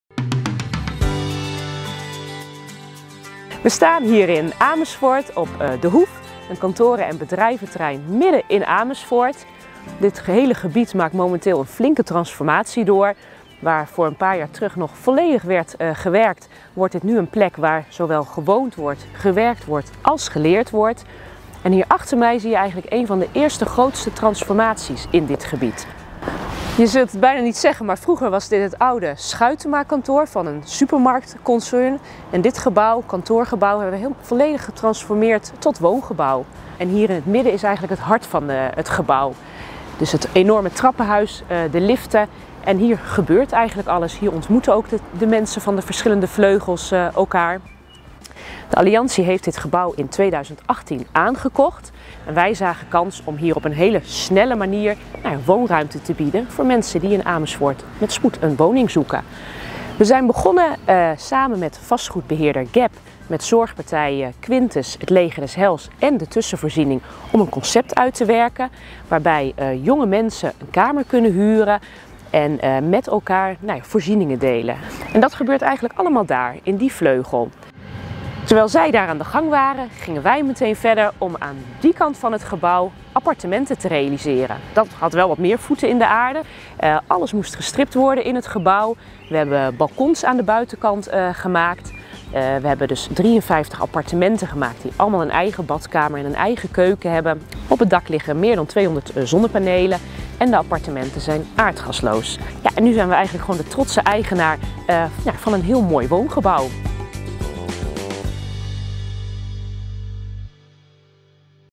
Transformatie van kantoorpand naar woningen.
Zo’n 6% van alle kantoor- en winkelruimte staat leeg. Kantoorpanden worden steeds vaker gebruikt om de woningnood aan te pakken. De Alliantie realiseert op deze manier regelmatig nieuwe woningen. Een belangrijke voorwaarde daarbij is dat de panden in een woonvriendelijke omgeving liggen. Het voormalige Schuitema-kantoor in het Hoefkwartier in Amersfoort veranderde in een wooncomplex met sociale huurwoningen, kamerwoningen en vrije sector koopappartementen. Transformatie van leegstaand vastgoed biedt niet alleen mensen met bescheiden inkomens en middeninkomens een thuis, het draagt ook bij aan de leefbaarheid in de wijk.